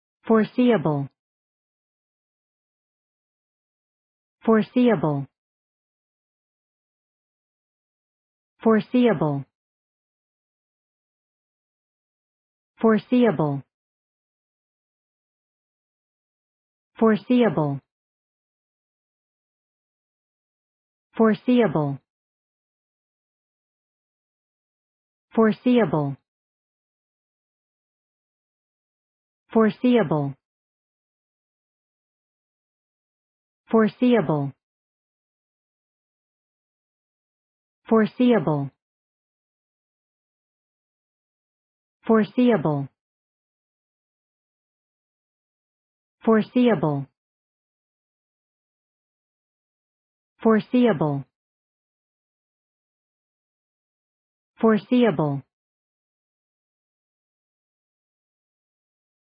Pronunciation of Foreseeable | Definition of Foreseeable
Foreseeable pronunciation | How to pronounce Foreseeable in English?
/fɔr`siʌbʌl/
Meaning of Foreseeable | What is Foreseeable?
(adjective) capable of being anticipated
What is Foreseeable | Foreseeable meaning and definition?